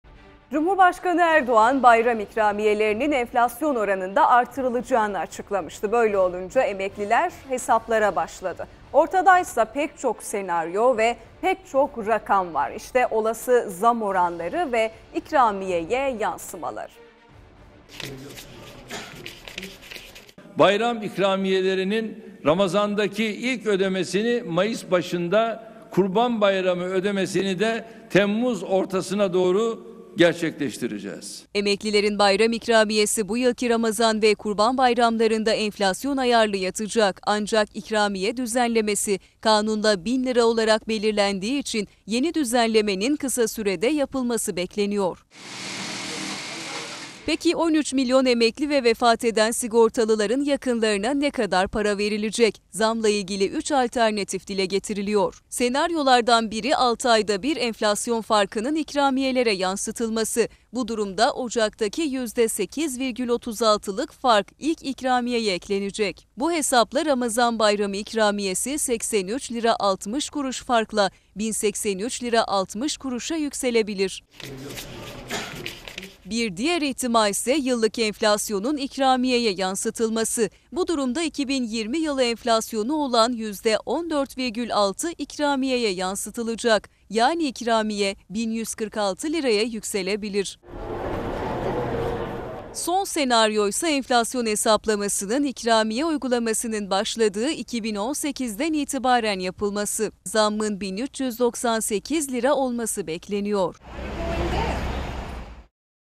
Bayram İkramiyesi Kaç Lira Olacak?
Cumhurbaşkanı Erdoğan Bayram İkramiyelerinin Enflasyon Oranında Artırılacağını Açıklamıştı. Böyle Olunca Emekliler Hesaplara Başladı. Ortadaysa Pek Çok Senaryo Ve Pek Çok Rakam Var. İşte Olası Zam Oranları Ve İkramiyeye Yansımaları.